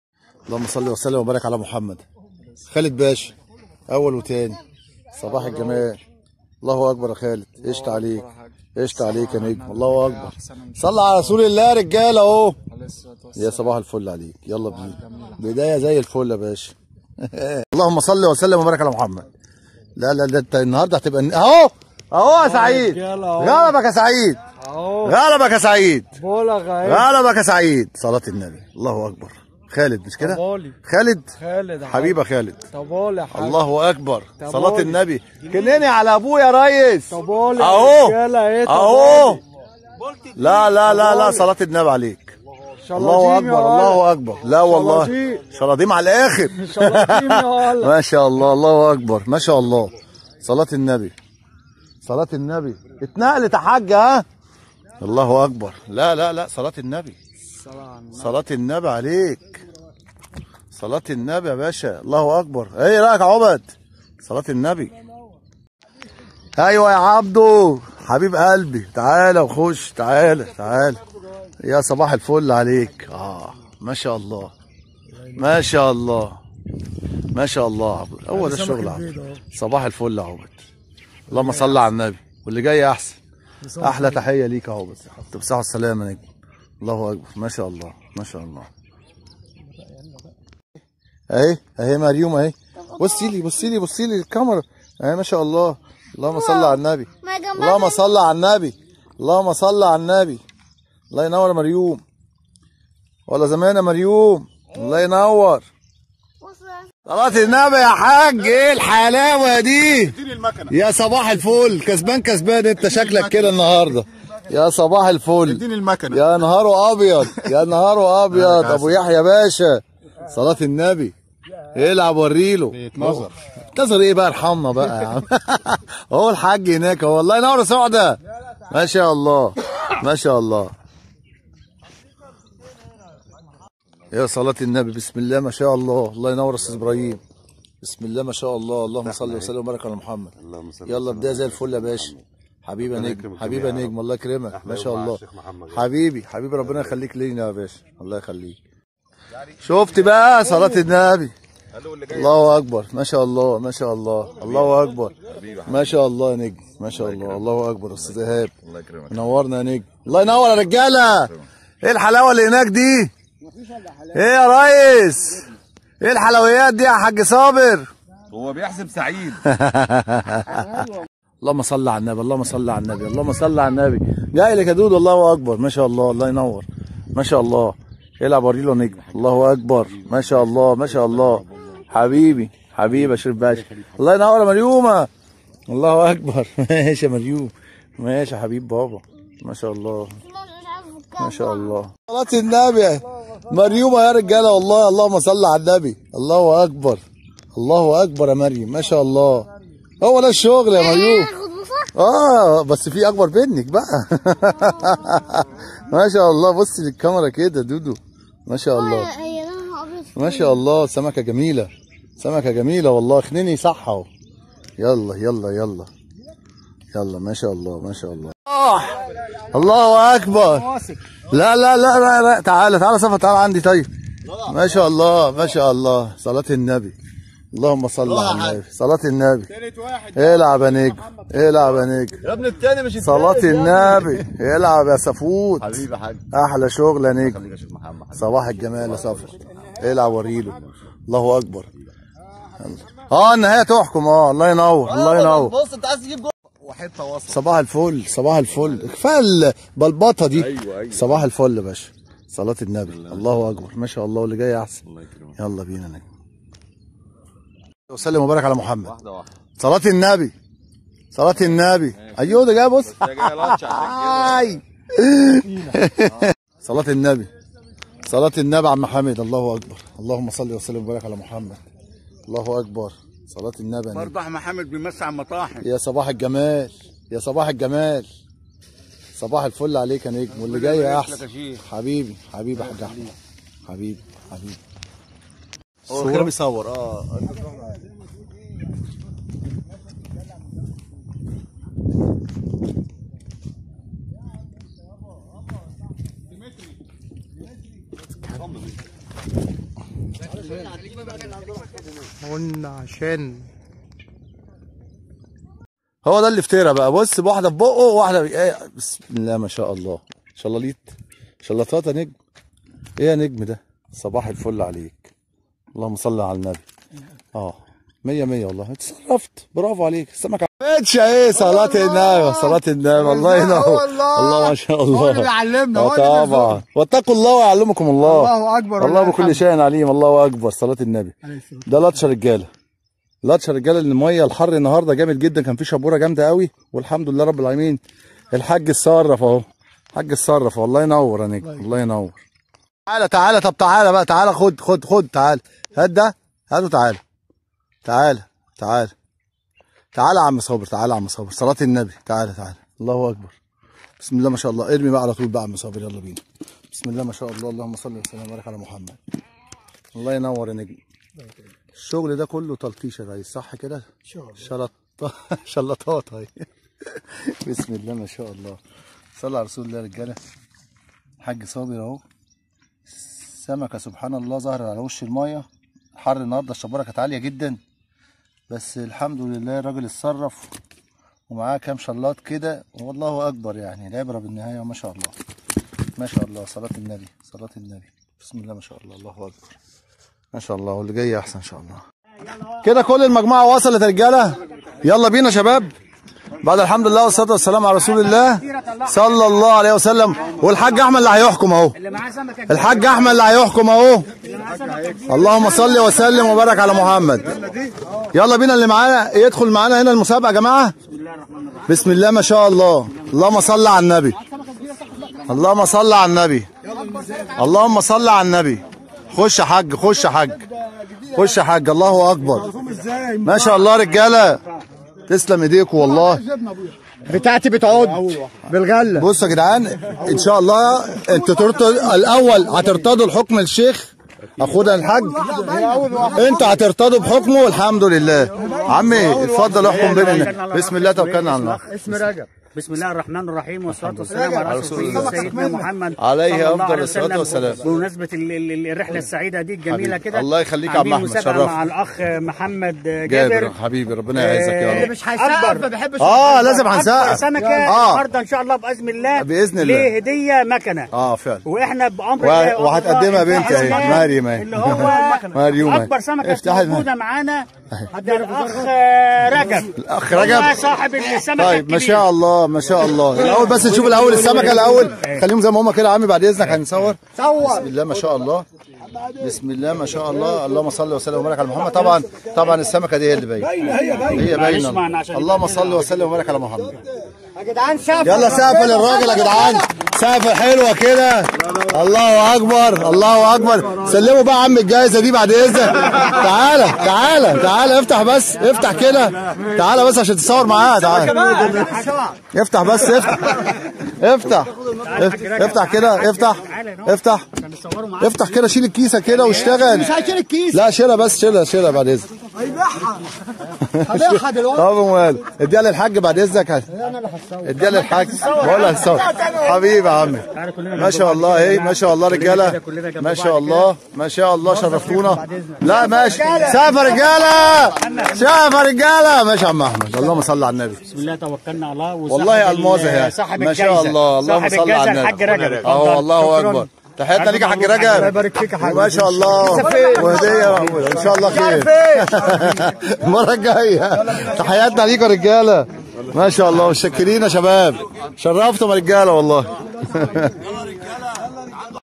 ٣٠ مارس، ٢٠٢٢ شاهد صيد السمك من بحيرة المية البيضاء ومن الفائز بالجائزة نهاية الفيديو
صيد السمك من بحيرة المية البيضاء ومن الفائز بالجائزة نهاية الفيديو